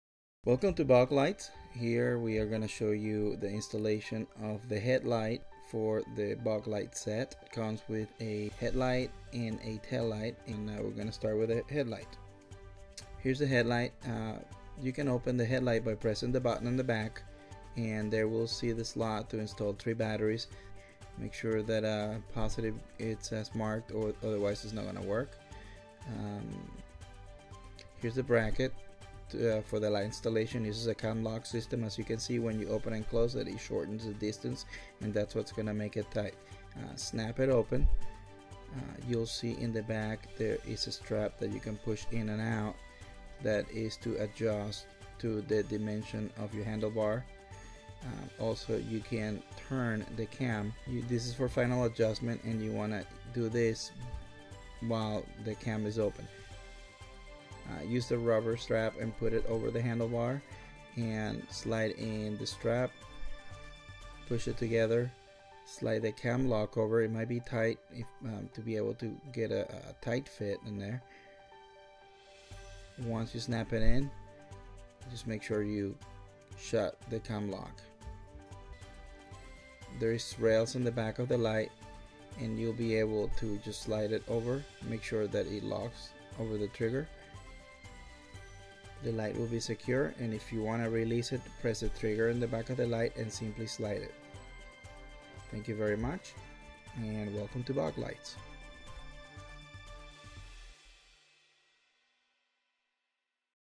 Bike Headlight Install Instructions - BoG Products bicycle Led light set
See how easy is to install your BöG Lights. simple steps on how to install bicycle lights. Enjoy your quality product.
This product is sold in amazon - find us under BoG Products